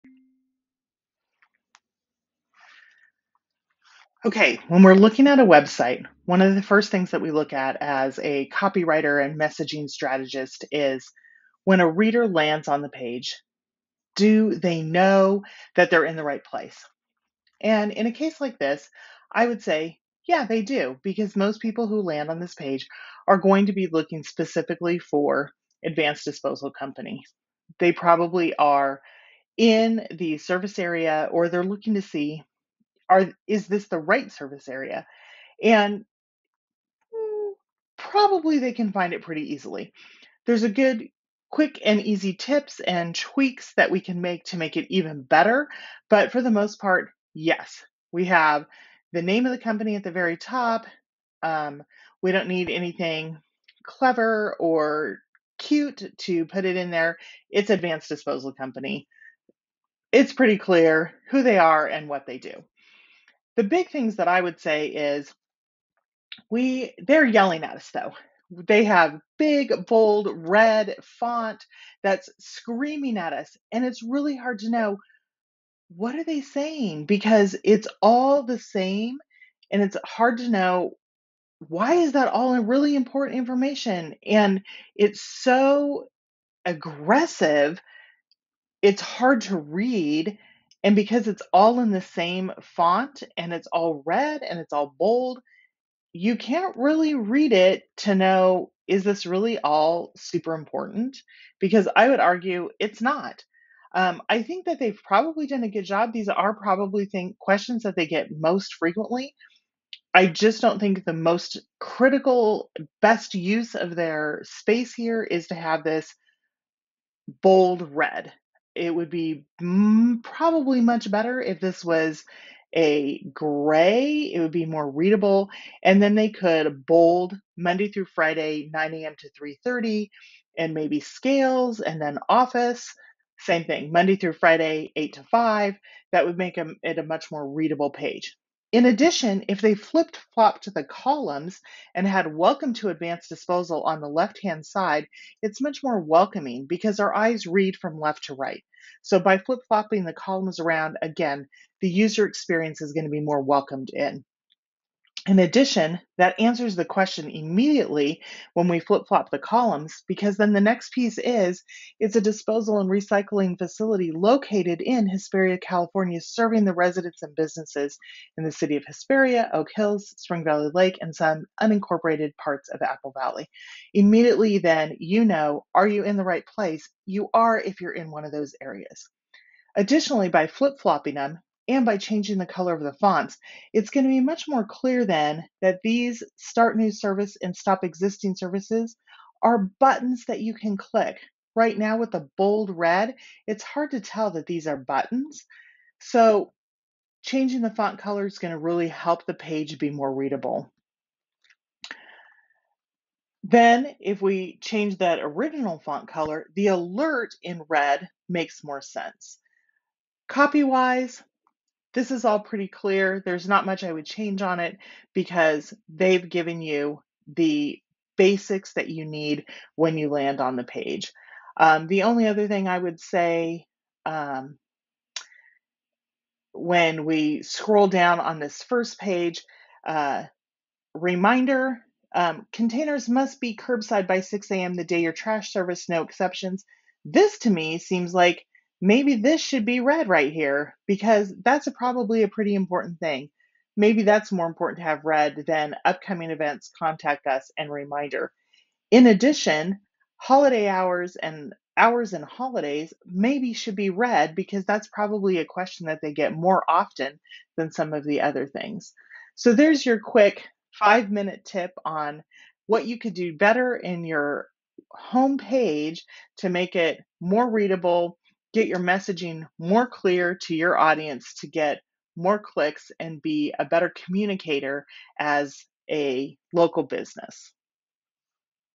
Trash Company Home Page Review
I never expect websites to scream at me but that's what this home page felt like. If you want your readers to feel welcome on your site, you want to avoid making these mistakes. This quick 5-minute home page audit breaks it all down.